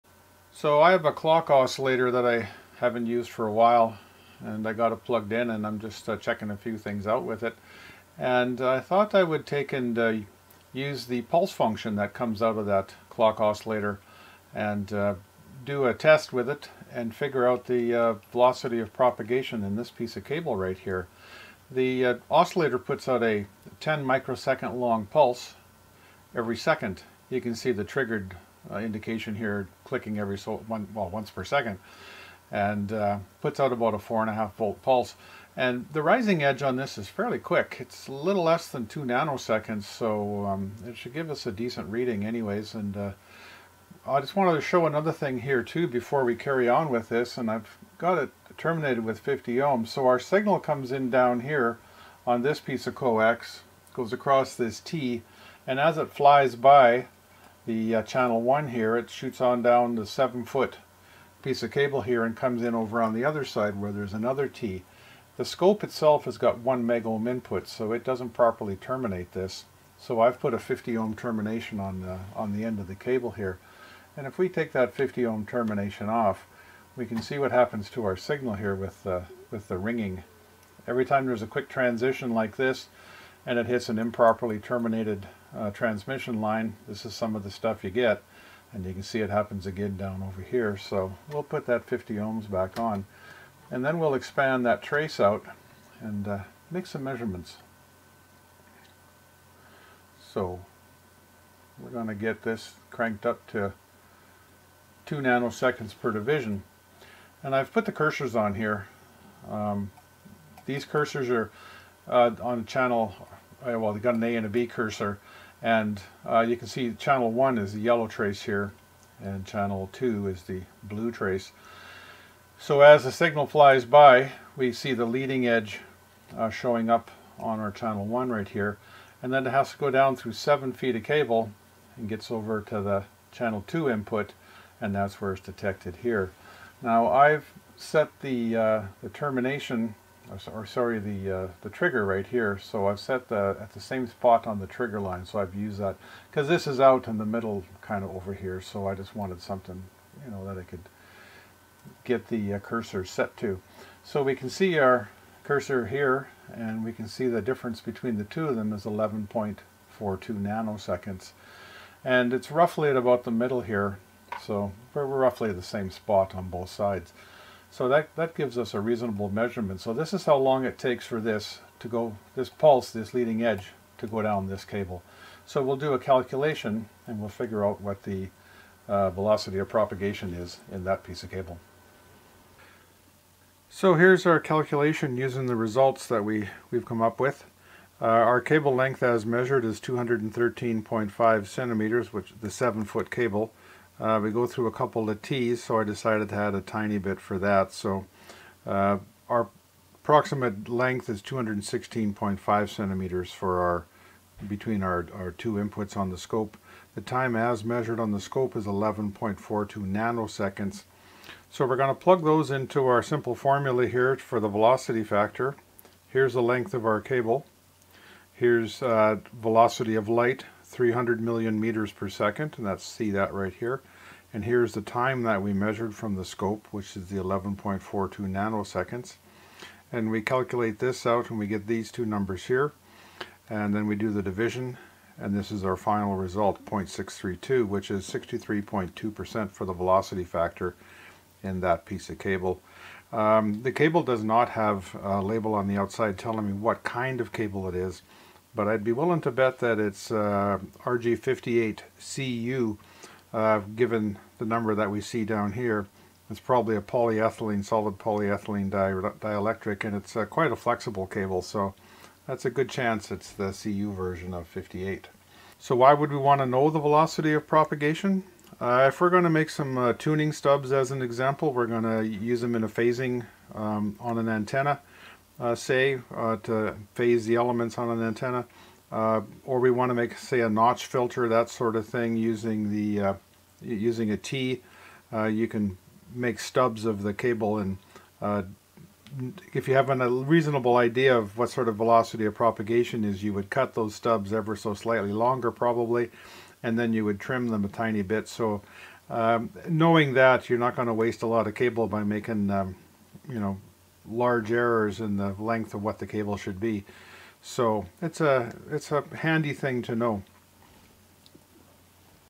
Velocity of Propagation In Unknown Coax Cable
Measuring the propagation delay in an unknown cable using an oscilloscope and the rising edge of a fast pulse.

Knowing the velocity of propagation is useful when phasing antenna elements, making delay lines, and making tuned stub notch filters for example.

0:00 Introduction
0:49 Improper Termination Demo
1:34 Measure Differential Timing
3:01 Calculation Using Measurements
4:08 Is It RG58C/U?
4:28 Why Measure VOP?